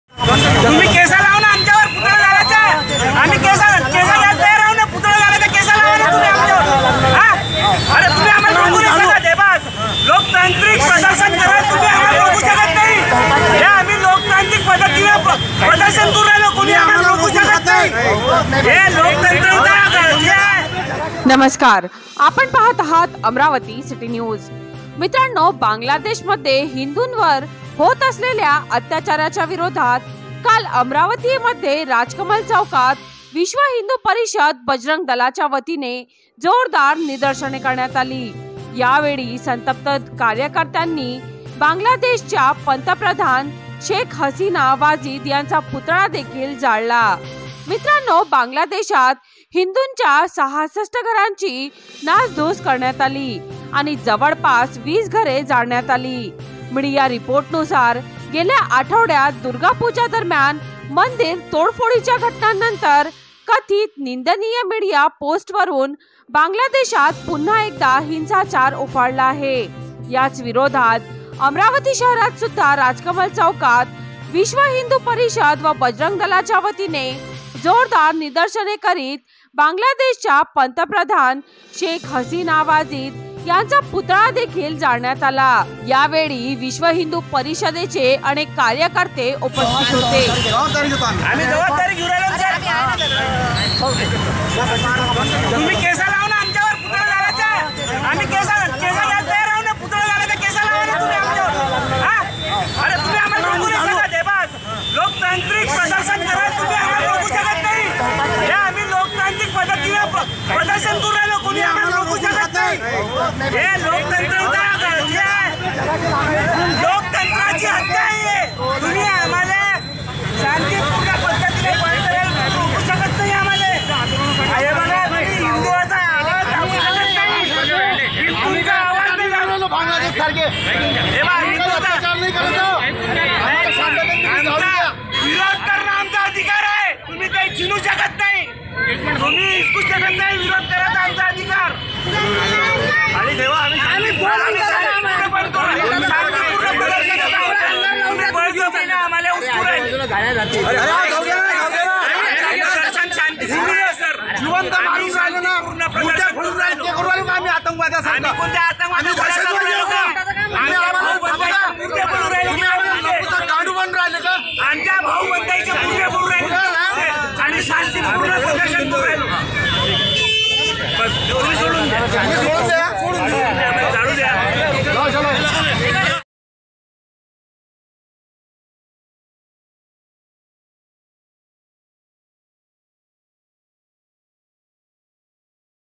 Amravati | अमरावतीत विश्व हिंदू परिषद व बजरंग दल आक्रामक,बांग्लादेशाच्या पंतप्रधांचा फुकला पुतळा।
Amravati | अमरावतीत विश्व हिंदू परिषद व
बजरंग दल आक्रामक,बांग्लादेशाच्या पंतप्रधांचा फुकला पुतळा।